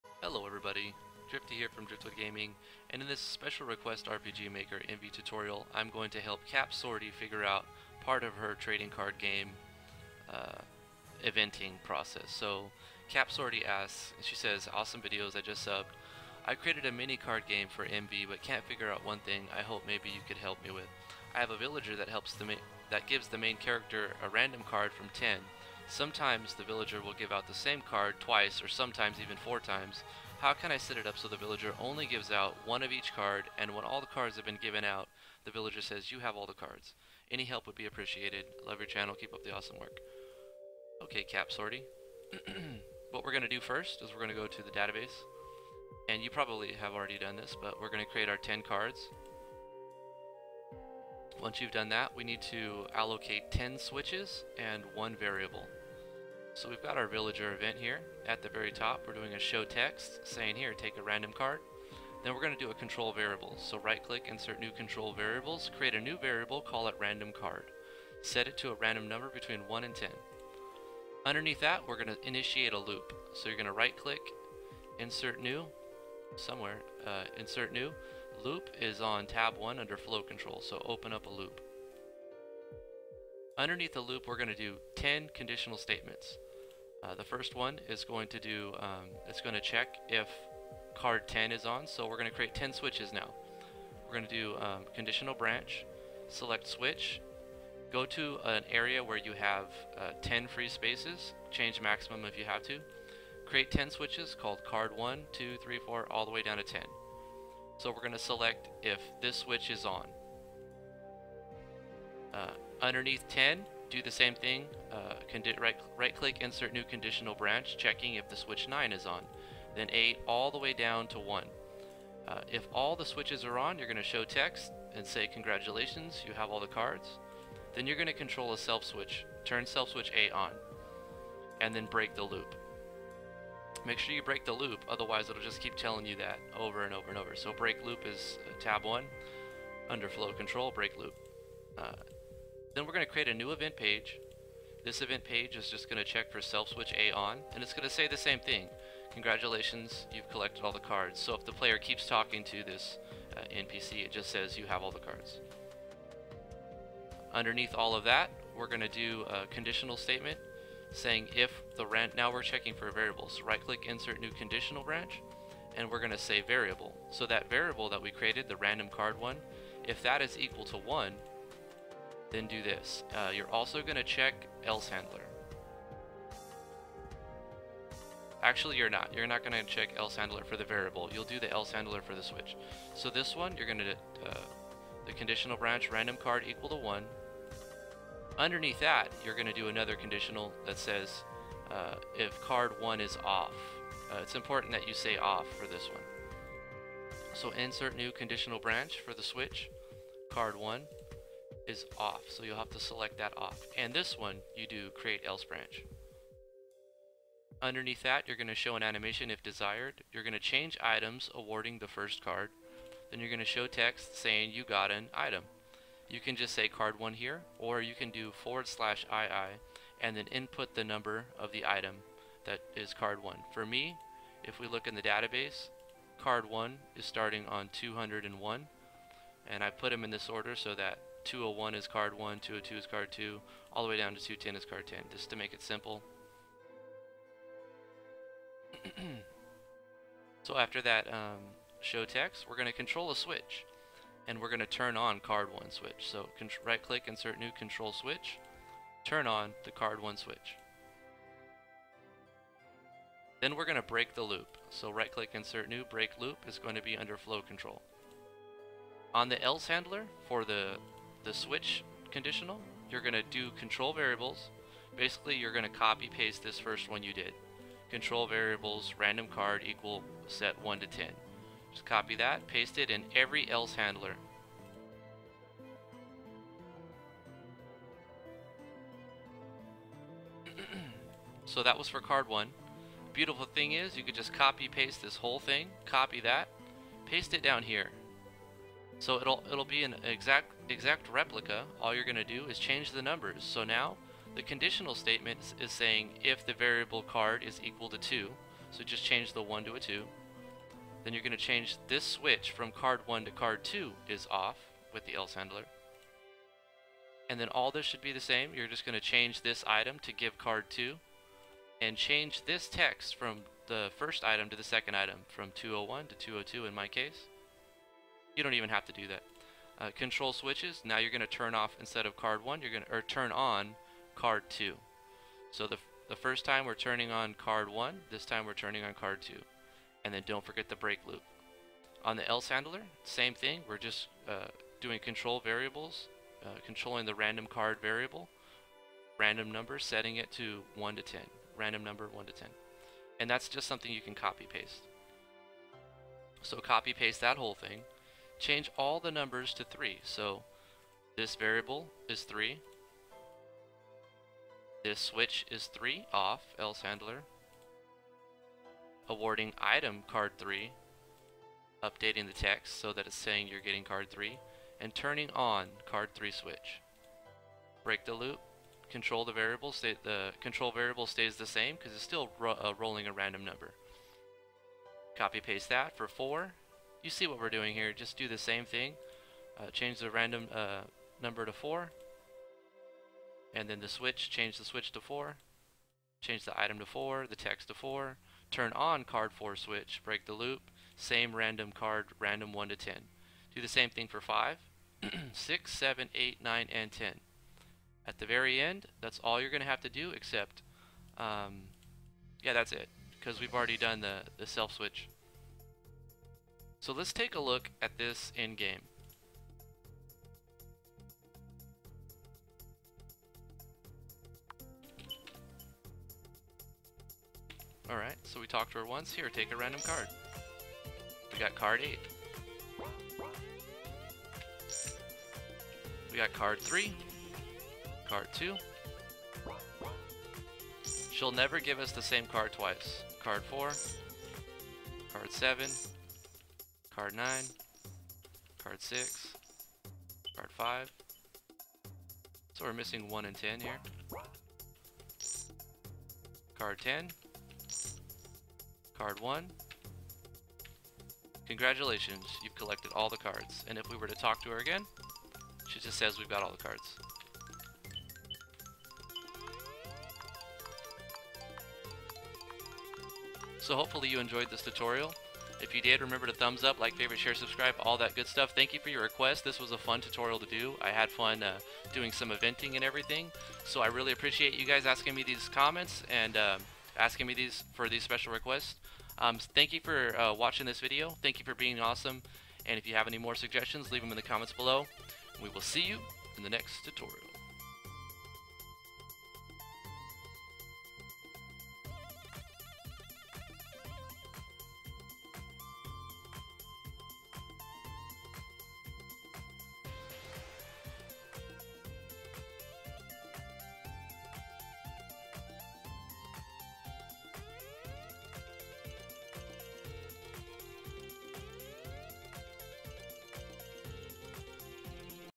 RPG Maker MV Eventing Part of a TCG
Hello everyone, in this special request RPG Maker MV tutorial I'll show you how to make an event that gives you one of a random 10 cards without giving you the same card twice. This was a fun challenge to figure out. :)